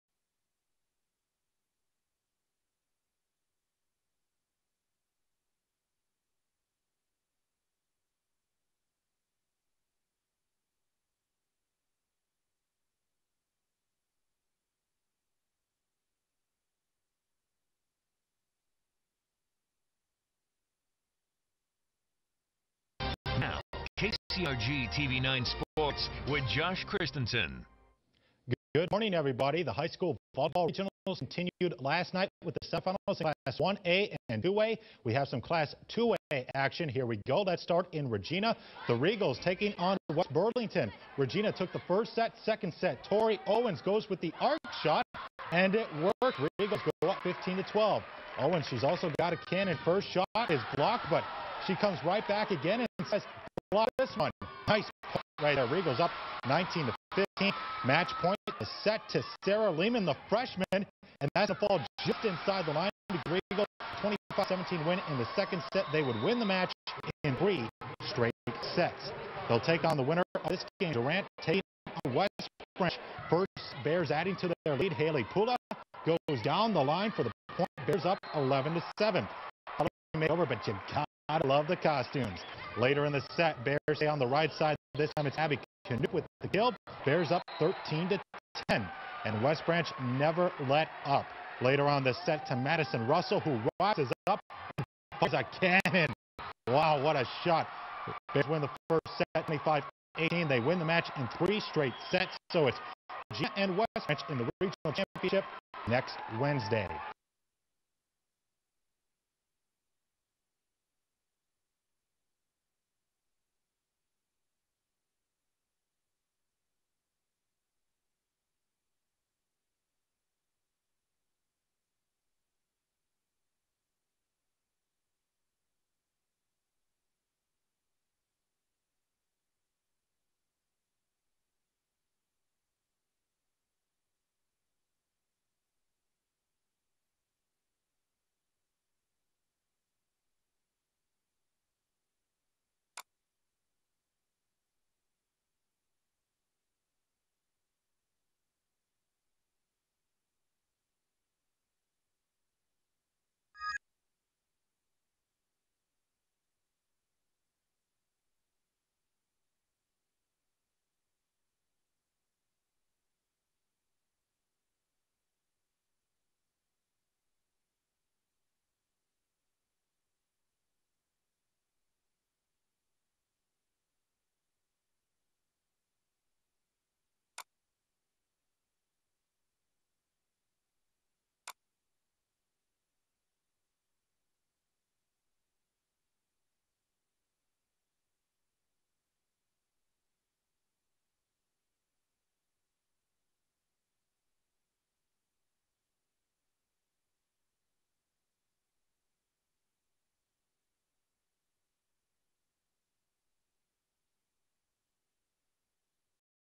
West Branch and Regina Move On To Regional Championship KCRG TV9 Cedar Rapids, Iowa News, Sports,
West Branch and Regina Move On To Regional VOLLEYBALL Championship